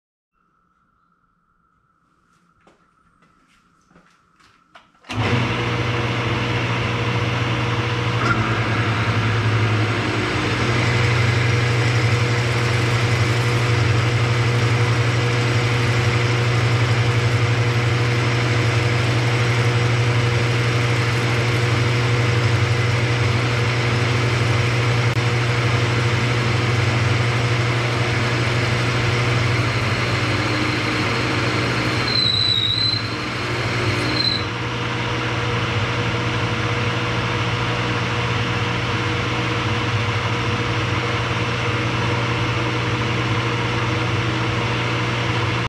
Milling Gate Stub from Stud Boss II
In the first of these two videos, I mentioned tool chatter and not knowing the cause.  Slowing the spindle speed from 1400 to 1000 rpm and adding a small amount of cutting oil solved the problem.  There a short squeal at the end but not tool chatter.

Using a two flute mill would probably help clear the chips but, since I don't have one, will continue with the 4-flute.

I am using a 1" diameter, 4 flute, center cutting, square end mill.  The stud bosses are 15/16" diameter.